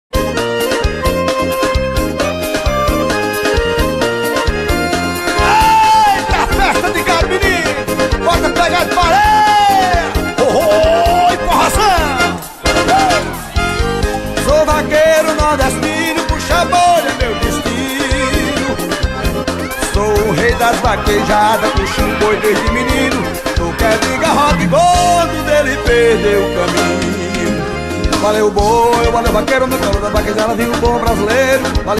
Boi Marroquino vai embora de novo
Boi Marroquino corre mais uma vez e vai EMBORA de novo.
Proprietário Branco de Zé de Dão